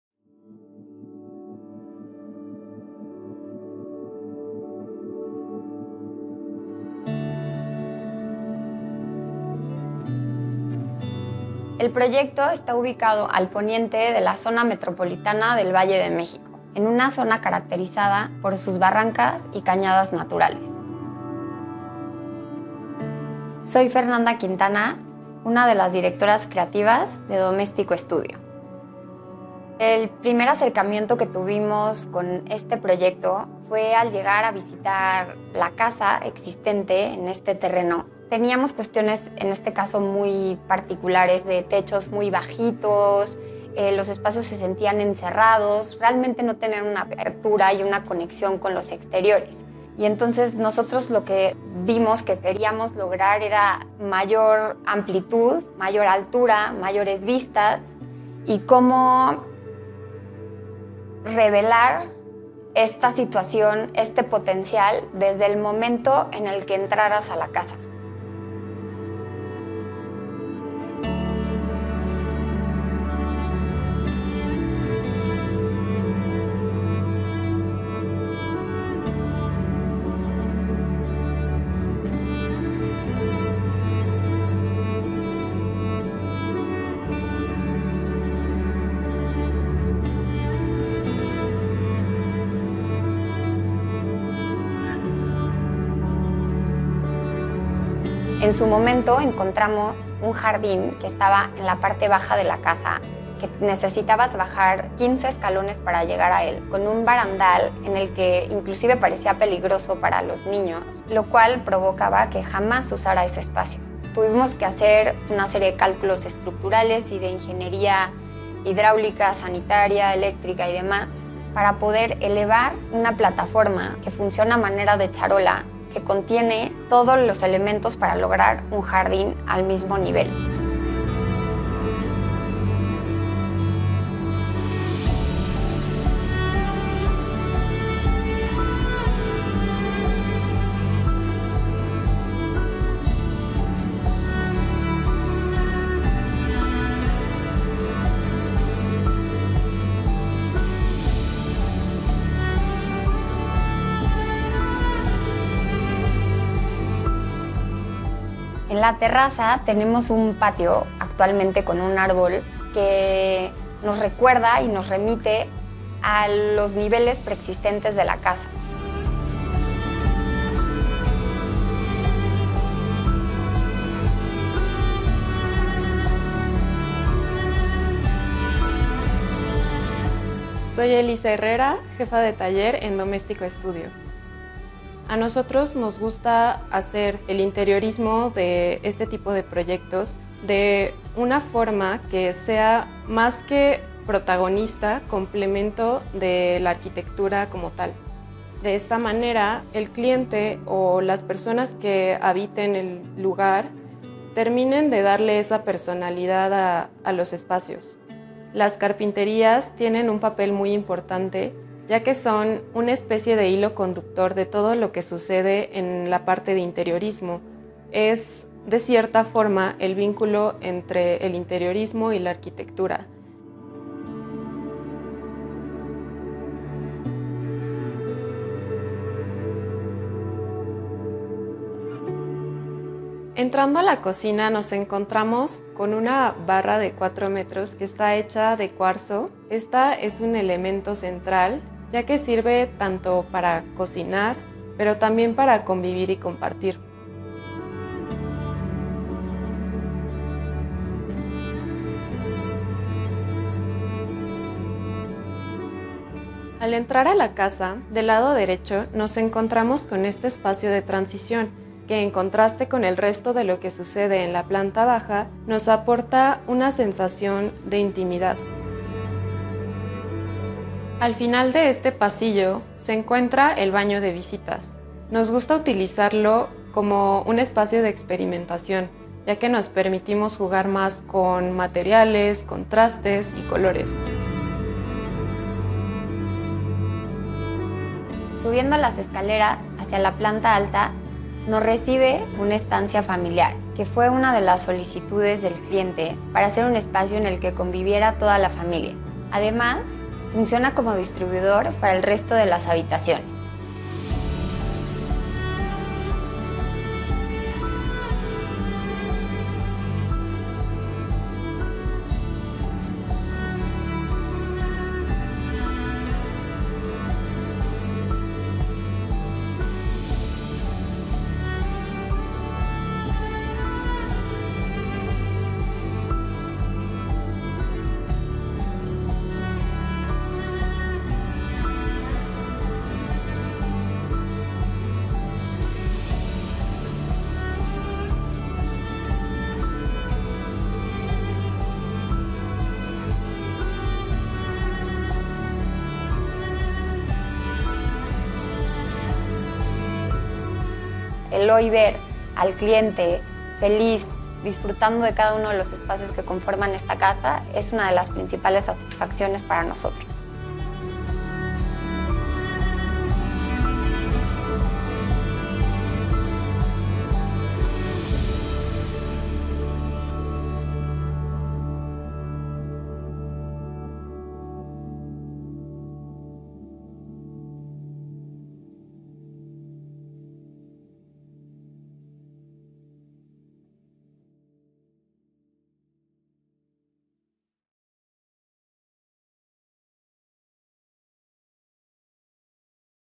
Una Bella Remodelación De Una Casa | Ep 04 OPEN PLAN | House Tour
Se solicitó a Doméstico Estudio un proyecto que reformara la casa preexistente de manera funcional y formal con la premisa de ampliar considerablemente las áreas comunes, sumado a la necesidad de incrementar el tamaño y número de habitaciones. Este fue el resultado que se logró; uno que permite a los usuarios disfrutar mejor de las vistas privilegiadas de las barrancas y cañadas naturales que ofrece este sitio.

Arquitectura: Doméstico Estudio
Producción Audiovisual: Open Plan

Open Plan nace con la finalidad de mostrar visiones únicas y singulares de diseño y arquitectura en México.

Capítulos:
00:00 Introducción
00:30 Brief
01:09 Planta Baja
01:38 Jardín
02:35 Terraza
03:00 Interiorismo
03:51 Cocina
04:30 Baño de Visitas
04:45 Planta Alta
04:51 Estancia Familiar
05:04 Habitaciones
05:51 Conclusión